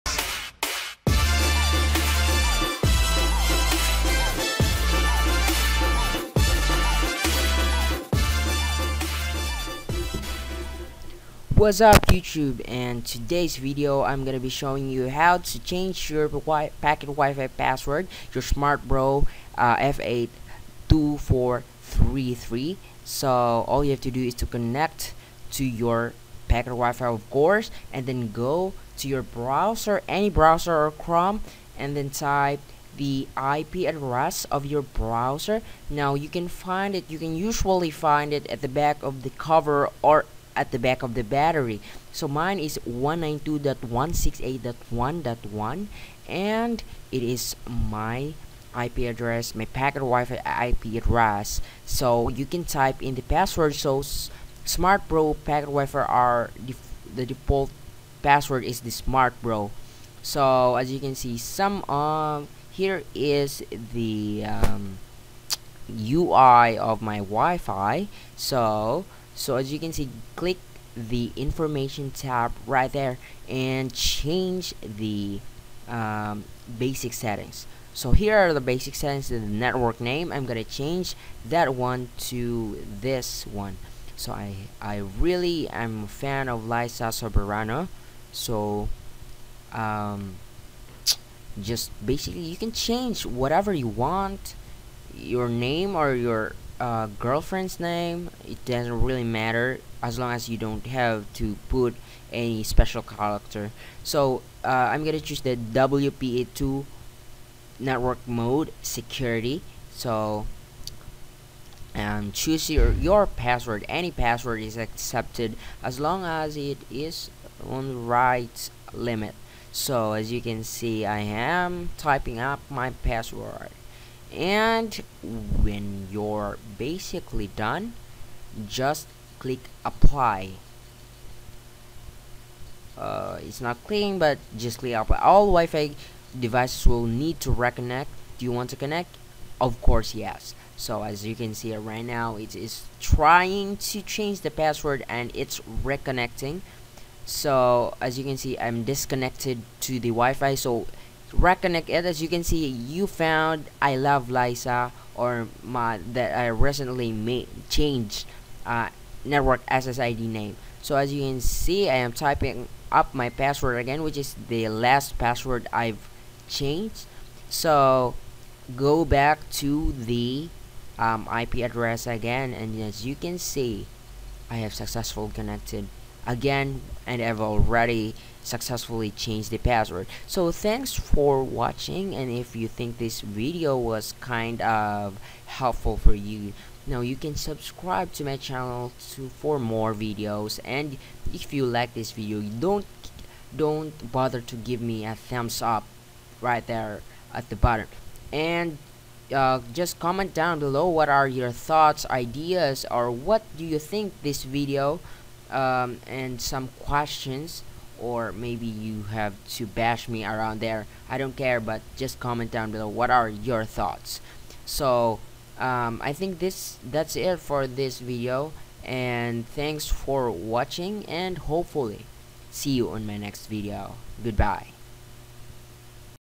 How to change/put SMART BRO POCKET WIFI password
Hey guys and im back, and in this video i will going to show you how to simply change of put your smart bro F82433  packet wifi .
Make sure that the Smart Bro SIM is correctly inserted in the Pocket WiFi and your computer is turned ON.

STEPS
Open the browser
Type the IP address provided on the product manual (ex. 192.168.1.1) and then press the Enter button
Type the password (default password: smartbro)
Click Login
Select Quick Settings
Click Next
Click Next again
Click Next
It is optional to change the SSID (Service Set Identifier) to personalize device name
Click Next
Adding an Encryption Mode helps protect your internect connection from unauthorized users
Select an Encryption Mode
Select WPA-PSK/WPA2-PSK
Type desired password
Click Next
Click the Apply button to finish